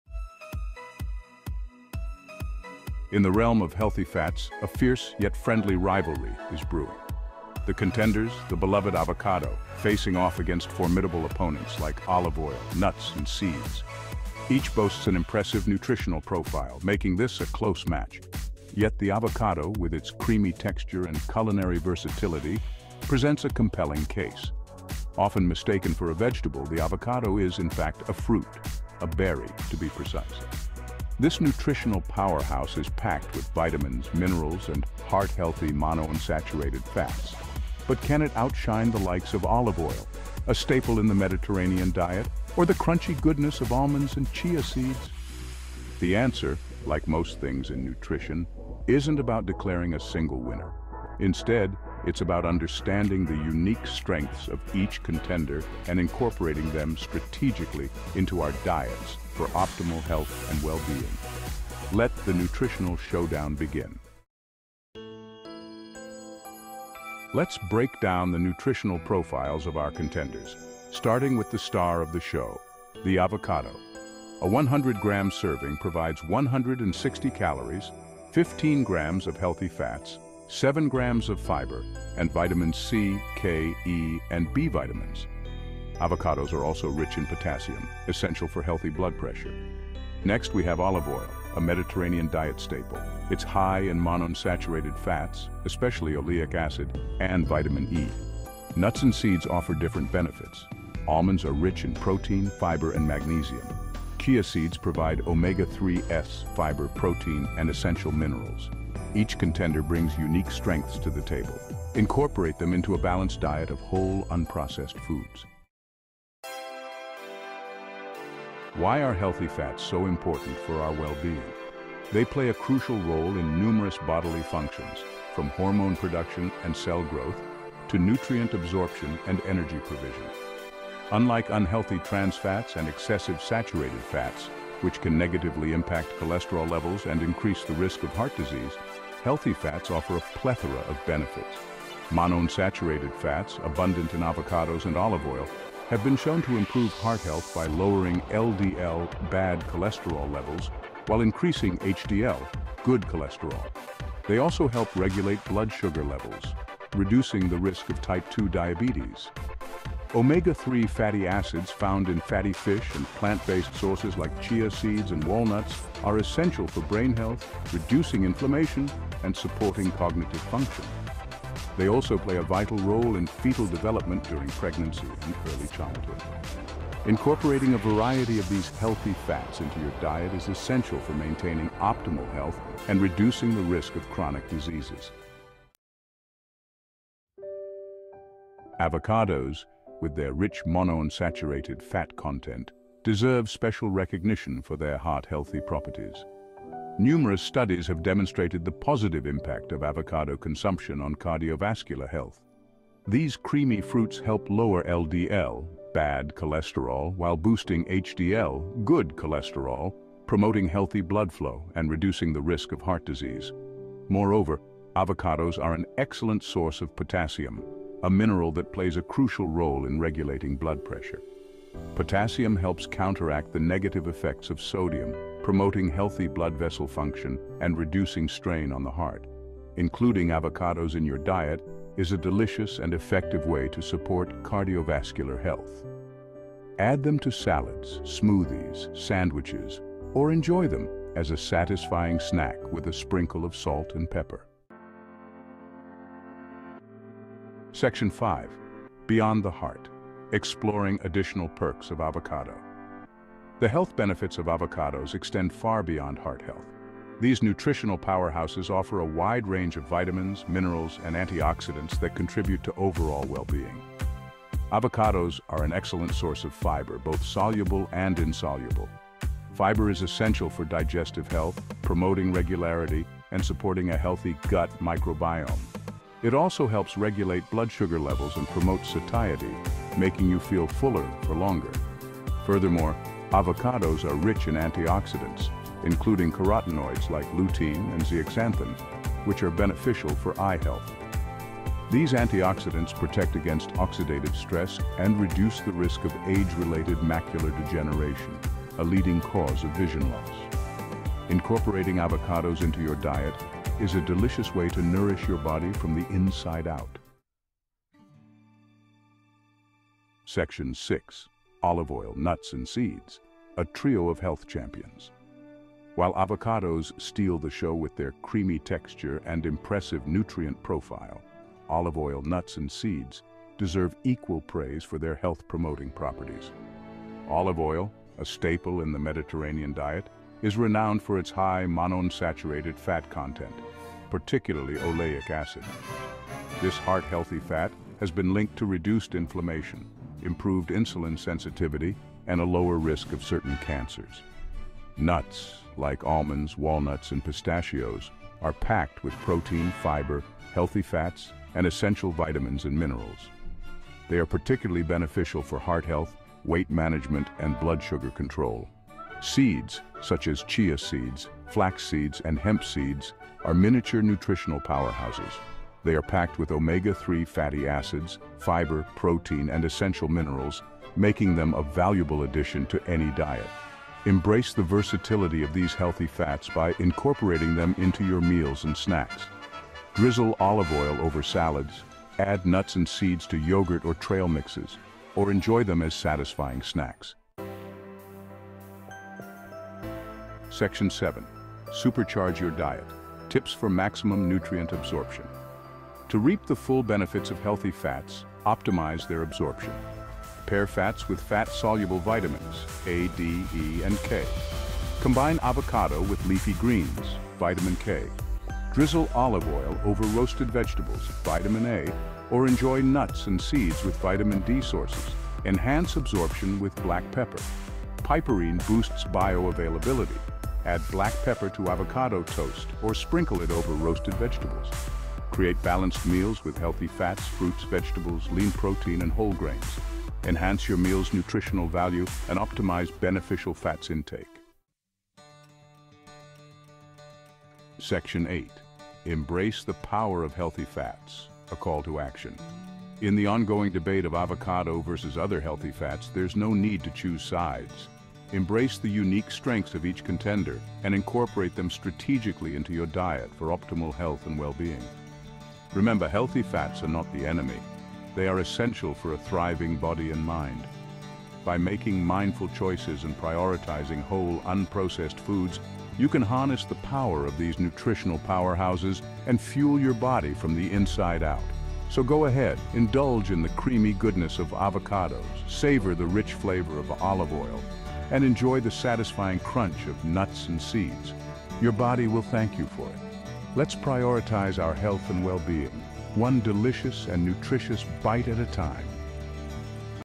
Avocado Vs Other Healthy Fats - Which Is Better for You?
Dive into the world of healthy fats with our latest video, where we explore the nutritional powerhouses: avocados, olive oil, nuts, and seeds. Uncover why these foods are not just good for you, but essential for optimal health.

We break down the benefits of each, from the heart-healthy monounsaturated fats in avocados and olive oil to the protein and omega-3s packed in nuts and seeds. Learn how to harness their unique strengths and incorporate them into your diet in delicious, effective ways.

Did you know that combining these fats with fat-soluble vitamins enhances nutrient absorption? Or that adding a pinch of black pepper can increase the bioavailability of certain nutrients? Join us as we discuss tips for maximizing the health benefits of these incredible foods.

OUTLINE:

00:00:00 A Worthy Opponent in the Healthy Fats Arena
00:01:04 Avocado vs. The Contenders
00:01:55 Unveiling the Benefits
00:03:05 A Love Story
00:04:04 Exploring Additional Perks of Avocado
00:05:07 A Trio of Health Champions
00:06:27 Tips for Maximum Nutrient Absorption